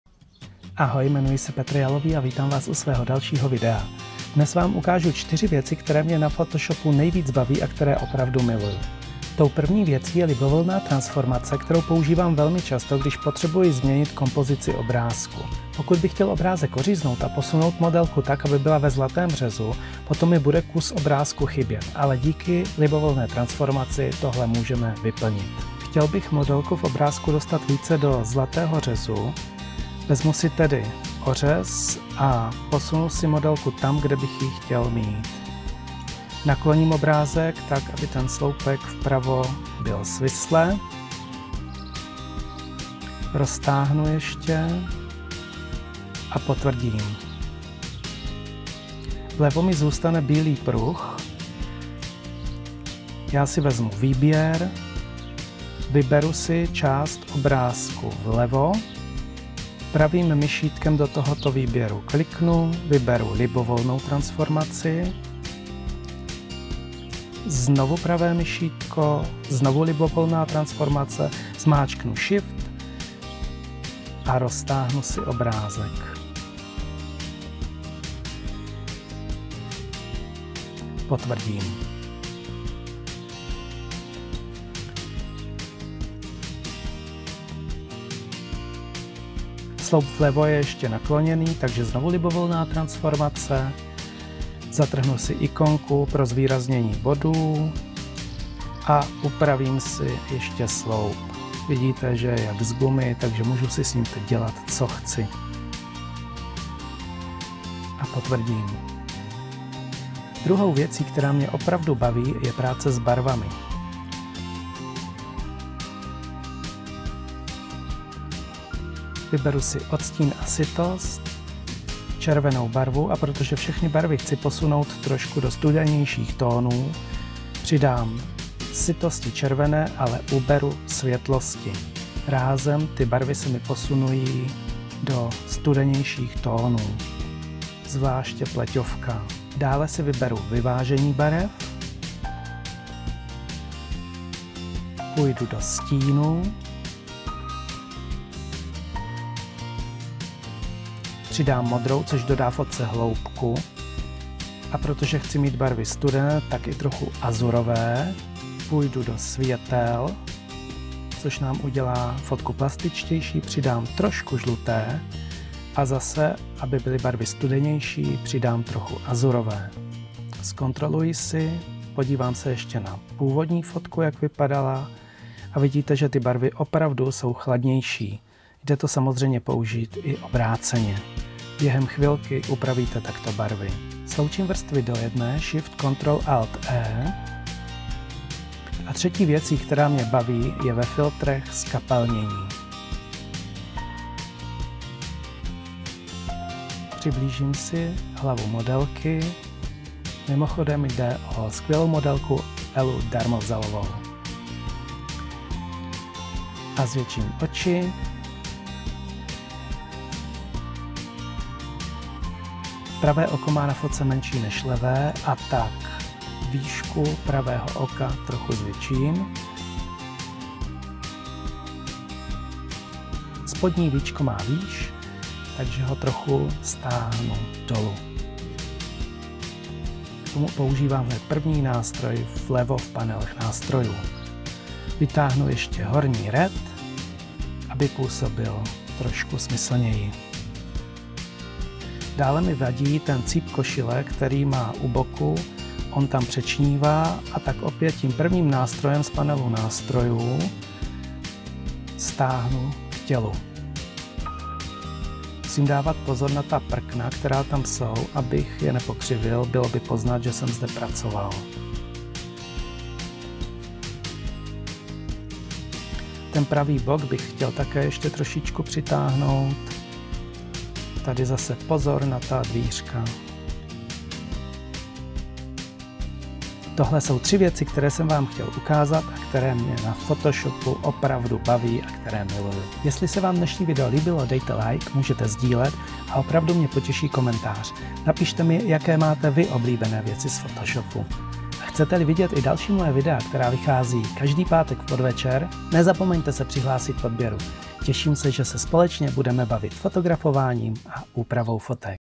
3 věci které na photoshopu miluji - photoshop tutorial - úprava fotek
3 věci které na photoshopu miluji - photoshop tutorial - úprava fotek
Dnes vám ukáži  tři jednoduché věci, které mi ve Photoshopu usnadňují práci na úpravě fotek.